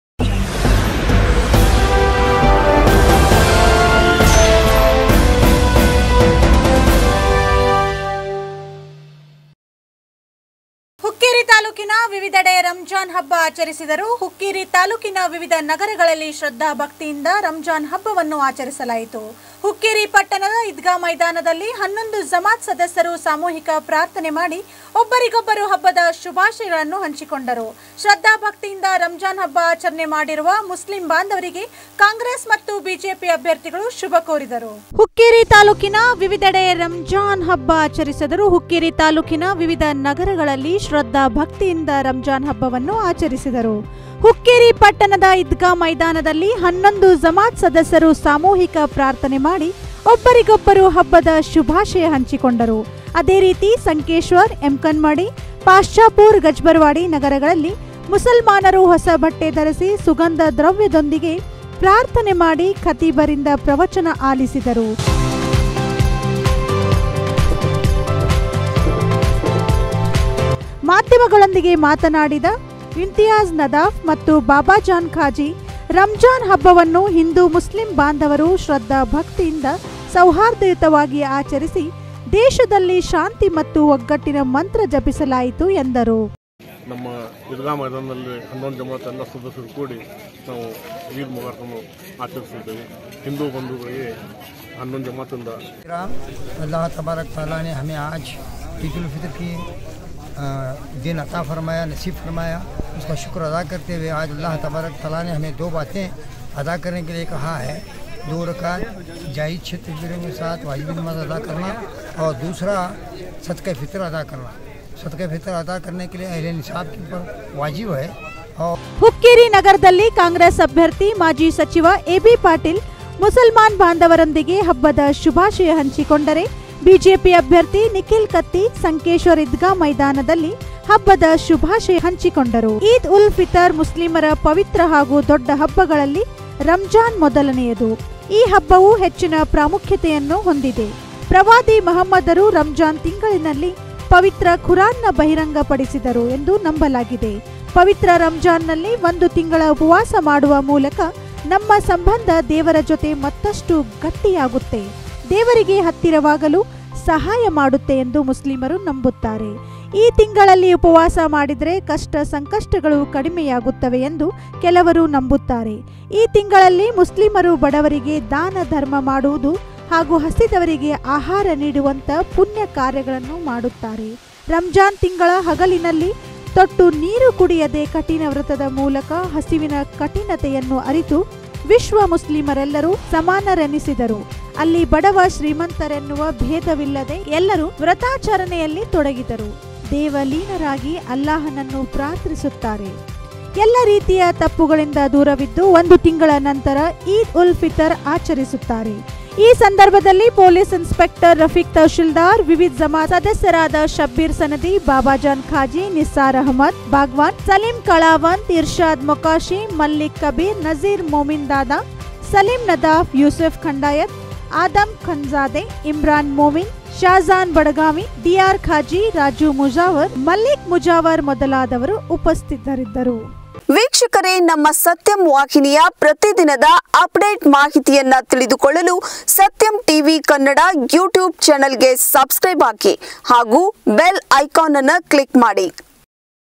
HUKKERI -ಶ್ರದ್ಧಾ ಭಕ್ತಿಯಿಂದ ರಂಜಾನ್ ಹಬ್ಬ ಆಚರಣೆ - ಕಾಂಗ್ರೆಸ್-ಬಿಜೆಪಿ ಅಭ್ಯರ್ಥಿಗಳಿಂದ ಶುಭ ಕೋರಿಕೆ.
SATYAM TV  KANNADA||